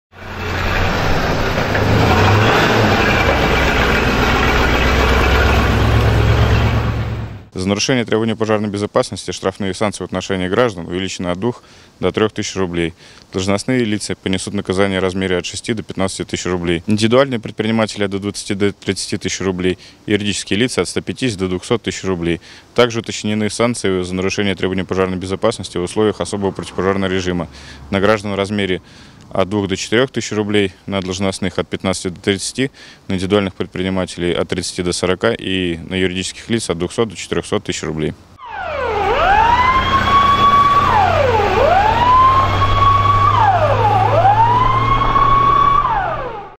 Десна-ТВ: Увеличены штрафы за нарушение пожарной безопасности
В соответствии с Федеральным законом от 28-го мая этого года в Кодекс об административных правонарушениях внесены изменения.